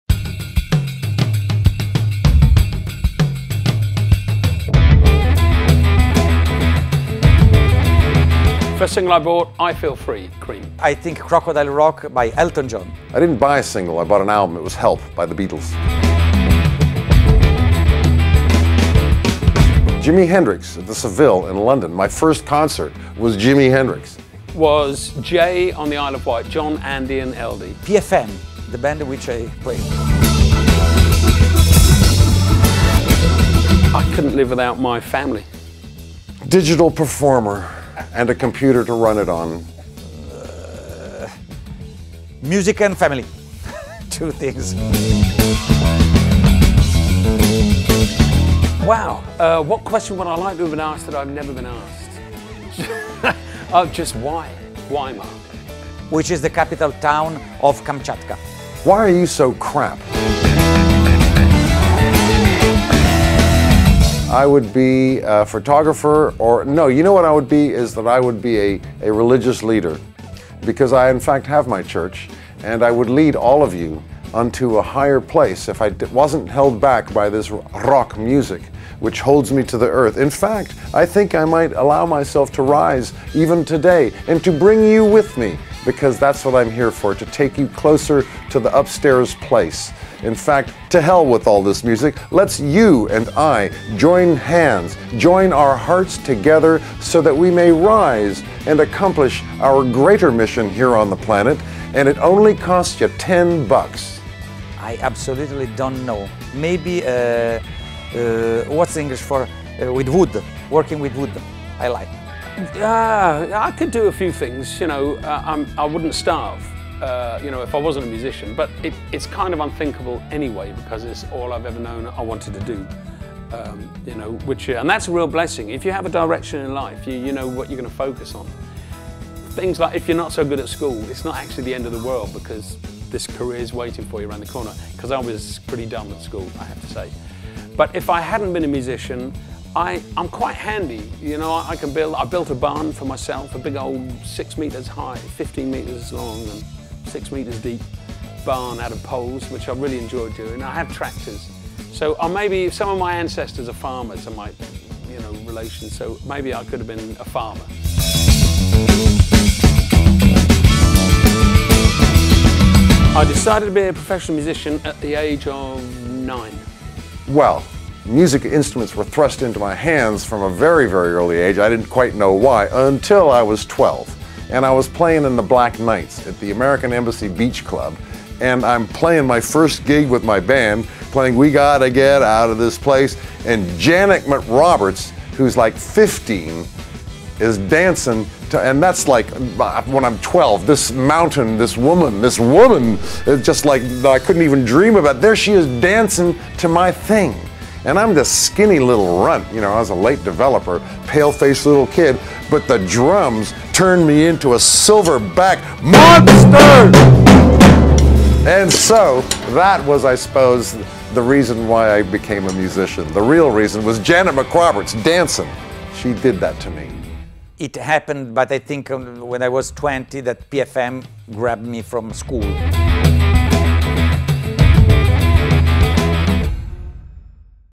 Gizmodrome - Rapid Fire Interview - Part 1
Three of the members of Gizmodrome take a little challenge with a rapid fire interview! This is part 1 of 3.